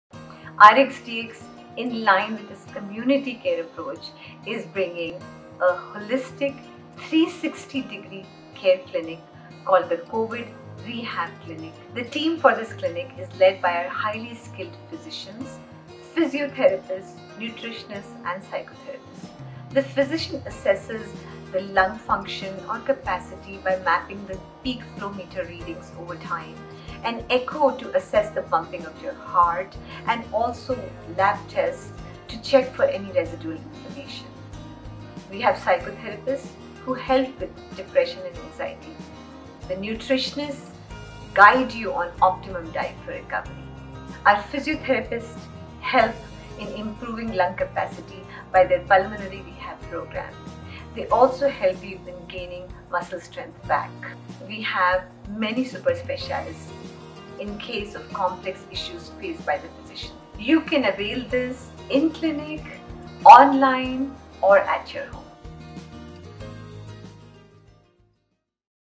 Long Covid Rehab | Post Covid Symptoms | Second Edition | RxDx Healthcare
Patients recovering from COVID have spoken of lingering fatigue, brain fog, memory and attention problems. Some have talked about shortness of breath, mild chest pain, and cough. For those with co-morbidities or advanced age, serious issues like blood clots have also been reported.

Ample rest and holistic healthcare support including symptomatic treatment and planned increase in activity are the key to rehabilitate one experiencing the post-viral-syndrome.